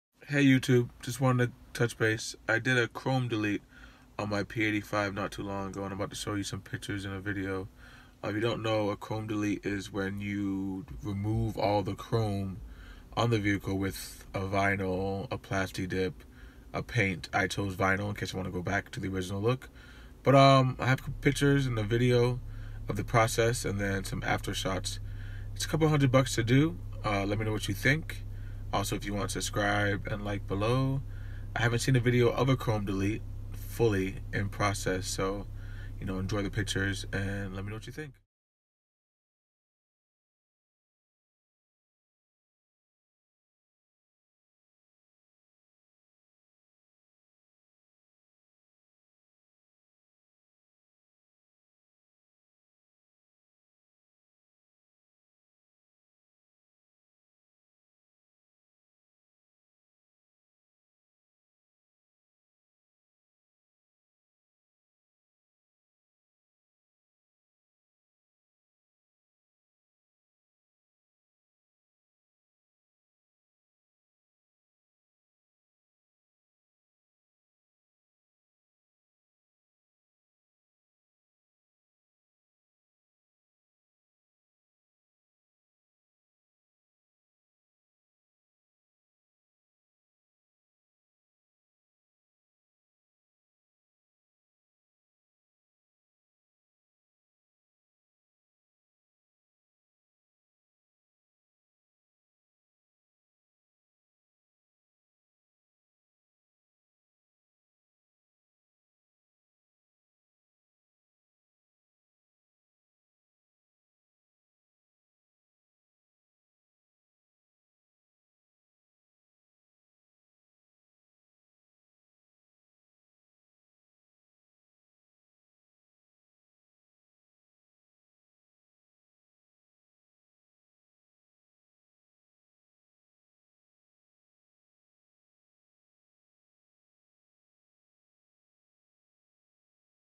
Tesla Model S P85 Chrome Delete Before:After
3m vinyl install on my chrome. 100% removable